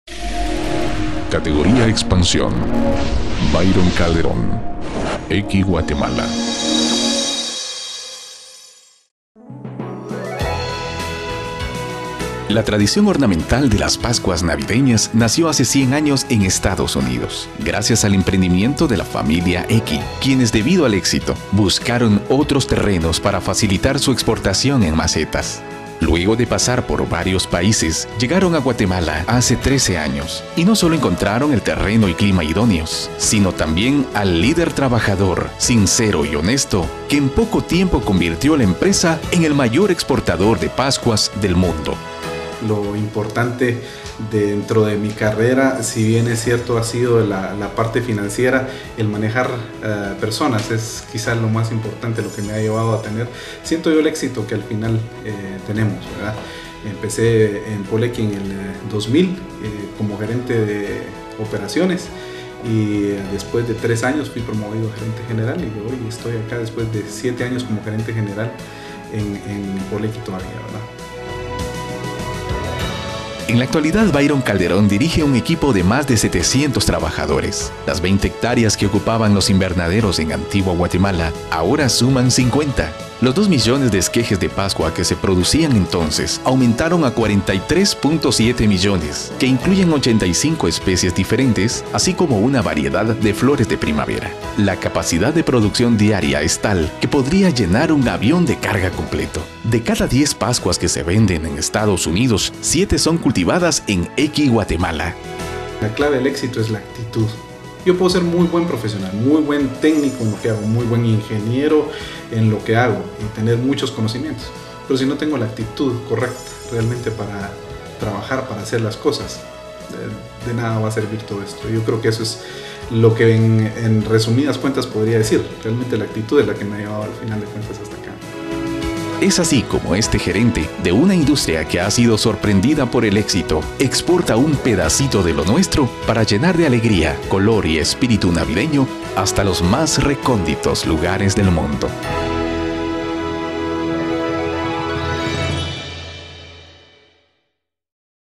BYRON CALDERÓN v corta DVD.mpg
Asociación de Gerentes de Guatemala
Nominado en "Excelencia Gerencial" en la categoría de Expansión